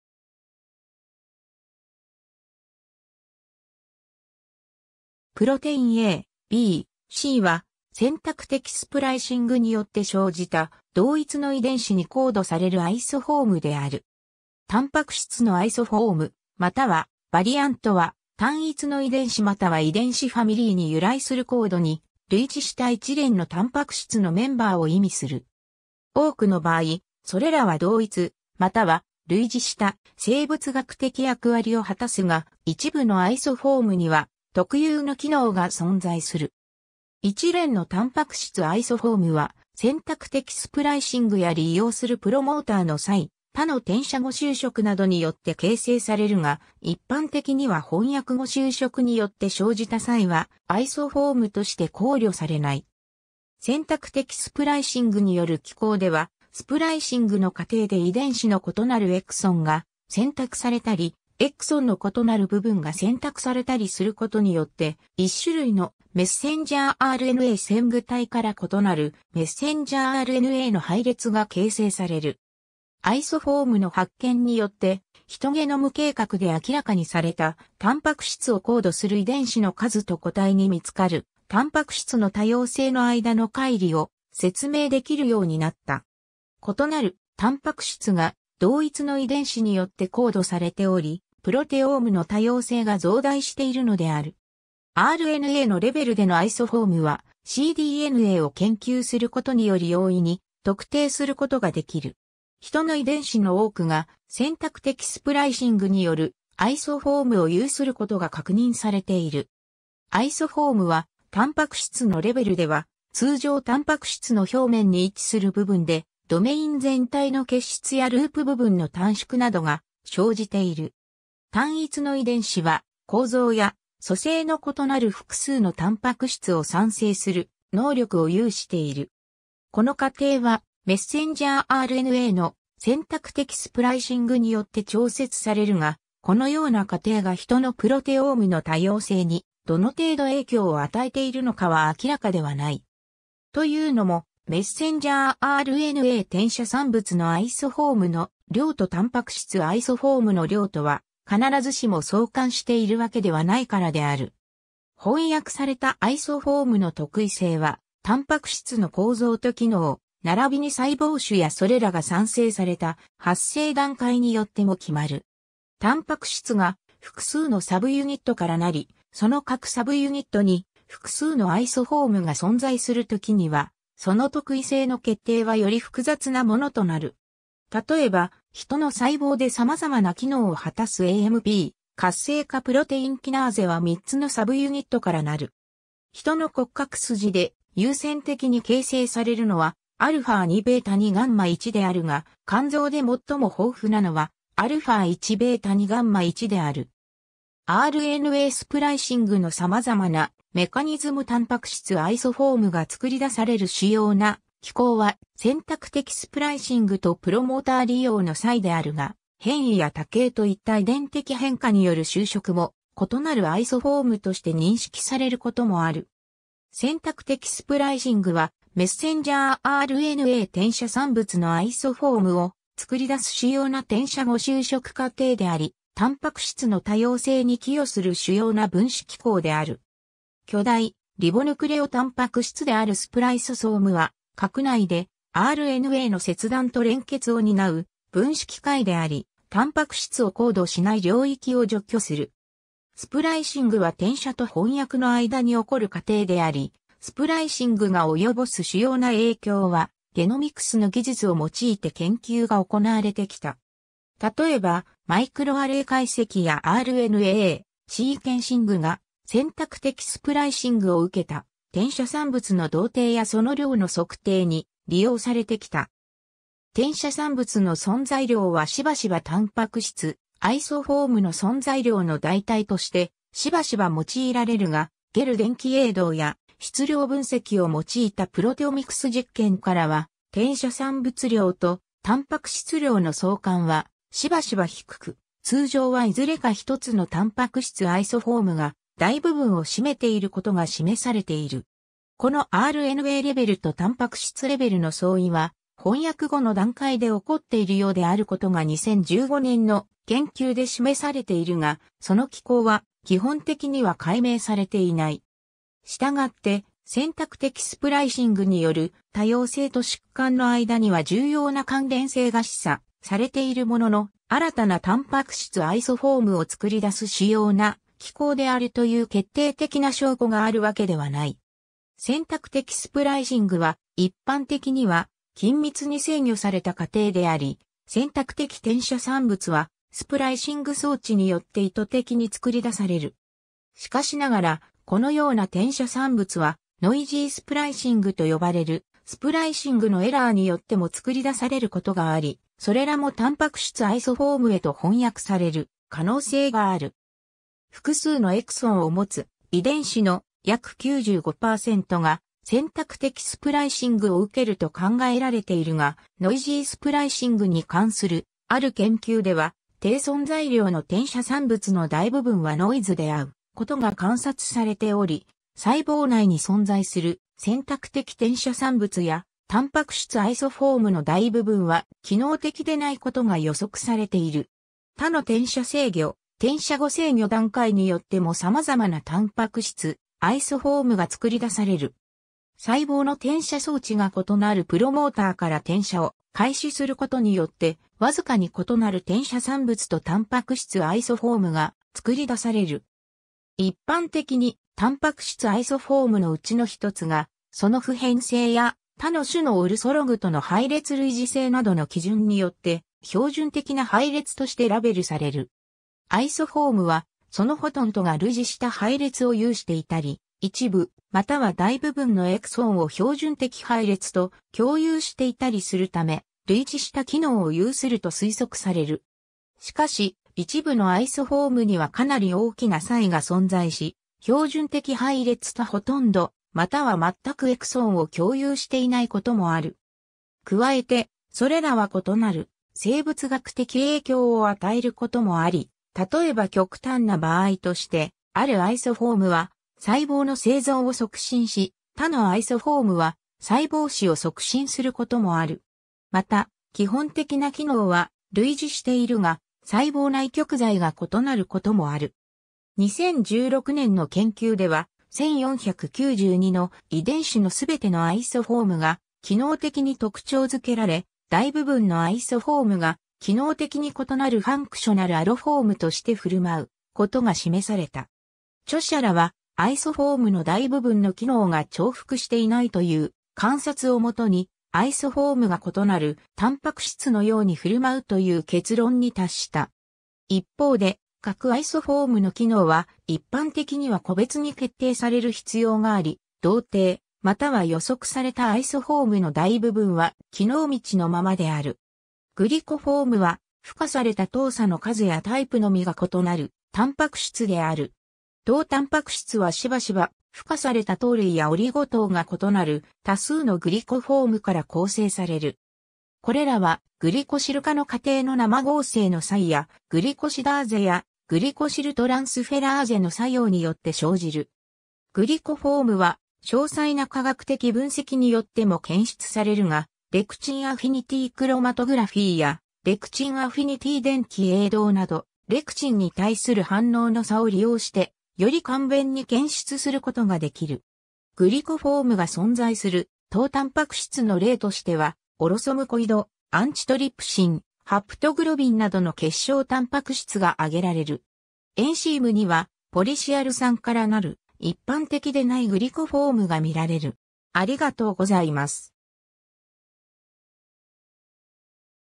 アイソフォーム
Protein A、B、Cは、選択的スプライシングによって生じた、同一の遺伝子にコードされるアイソフォームである。
 タンパク質のアイソフォーム（英: Protein isoform）またはバリアント（variant）は、単一の遺伝子または遺伝子ファミリーに由来する高度に類似した一連のタンパク質のメンバーを意味する。
多くの場合、それらは同一または類似した生物学的役割を果たすが、一部のアイソフォームには特有の機能が存在する。
一連のタンパク質アイソフォームは、選択的スプライシングや利用するプロモーターの差異、他の転写後修飾などによって形成されるが、一般的には翻訳後修飾によって生じた差異はアイソフォームとして考慮されない。
 選択的スプライシングによる機構では、スプライシングの過程で遺伝子の異なるエクソン（タンパク質コード領域）が選択されたり、エクソンの異なる部分が選択されたりすることによって、1種類のmRNA前駆体から異なるmRNAの配列が形成される。
 アイソフォームの発見によって、ヒトゲノム計画で明らかにされたタンパク質をコードする遺伝子の数と個体に見つかるタンパク質の多様性の間の乖離を説明できるようになった。
異なるタンパク質が同一の遺伝子によってコードされており、プロテオームの多様性が増大しているのである。
RNAのレベルでのアイソフォームは、cDNAを研究することにより容易に特定することができる。
ヒトの遺伝子の多くが選択的スプライシングによるアイソフォームを有することが確認されている。
アイソフォームはタンパク質のレベルでは、通常タンパク質の表面に位置する部分で、ドメイン全体の欠失やループ部分の短縮などが生じている。
 単一の遺伝子は、構造や組成の異なる複数のタンパク質を産生する能力を有している。
この過程はmRNAの選択的スプライシングによって調節されるが、このような過程がヒトのプロテオームの多様性にどの程度影響を与えているのかは明らかではない。
というのも、mRNA転写産物のアイソフォームの量とタンパク質アイソフォームの量とは必ずしも相関しているわけではないからである。
 翻訳されたアイソフォームの特異性は、タンパク質の構造と機能、ならびに細胞種やそれらが産生された発生段階によっても決まる。
タンパク質が複数のサブユニットからなり、その各サブユニットに複数のアイソフォームが存在するときには、その特異性の決定はより複雑なものとなる。
 例えば、ヒトの細胞でさまざまな機能を果たすAMP活性化プロテインキナーゼ（AMPK）は3つのサブユニットからなる。
 ヒトの骨格筋で優先的に形成されるのはα2β2γ1であるが、肝臓で最も豊富なのはα1β2γ1である。
 RNAスプライシングのさまざまなメカニズム タンパク質アイソフォームが作り出される主要な機構は選択的スプライシングとプロモーター利用の差異であるが、変異や多型といった遺伝的変化による修飾も異なるアイソフォームとして認識されることもある。
 選択的スプライシングはmRNA転写産物のアイソフォームを作り出す主要な転写後修飾過程であり、タンパク質の多様性に寄与する主要な分子機構である。
巨大リボヌクレオタンパク質であるスプライソソームは、核内でRNAの切断と連結を担う分子機械であり、タンパク質をコードしない領域（イントロン）を除去する。
 スプライシングは転写と翻訳の間に起こる過程であり、スプライシングが及ぼす主要な影響はゲノミクスの技術を用いて研究が行われてきた。
例えば、マイクロアレイ解析やRNAシーケンシングが選択的スプライシングを受けた転写産物の同定やその量の測定に利用されてきた。
転写産物の存在量はしばしばタンパク質アイソフォームの存在量の代替としてしばしば用いられるが、ゲル電気泳動や質量分析を用いたプロテオミクス実験からは、転写産物量とタンパク質量の相関はしばしば低く、通常はいずれか1つのタンパク質アイソフォームが大部分を占めていることが示されている。
このRNAレベルとタンパク質レベルの相違は翻訳後の段階で起こっているようであることが2015年の研究で示されているが、その機構は基本的には解明されていない。
したがって、選択的スプライシングによる多様性と疾患の間には重要な関連性が示唆されているものの、新たなタンパク質アイソフォームを作り出す主要な機構であるという決定的な証拠があるわけではない。
 選択的スプライシングは一般的には緊密に制御された過程であり、選択的転写産物はスプライシング...